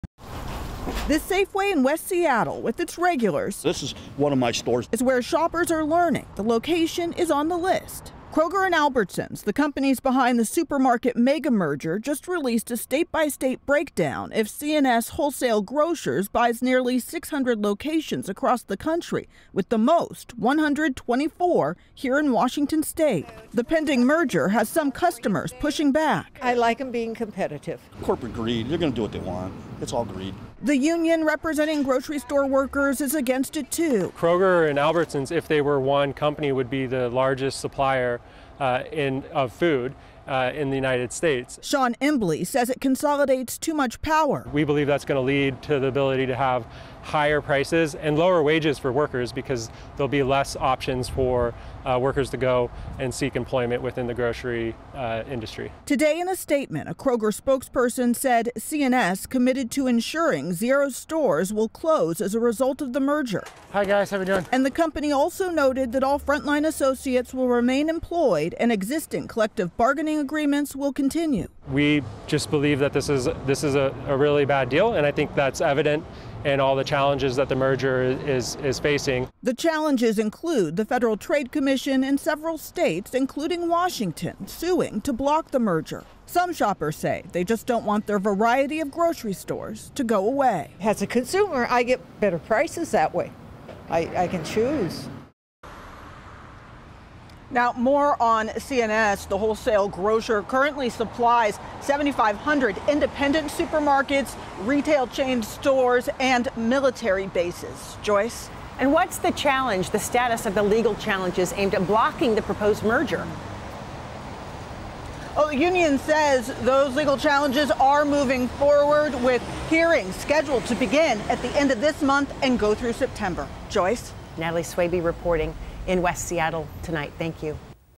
Union, customers respond to possible sale of 124 Kroger, Albertsons stores in Washington
The list includes Haggen, Safeway and QFC grocery stores in dozens of Washington cities.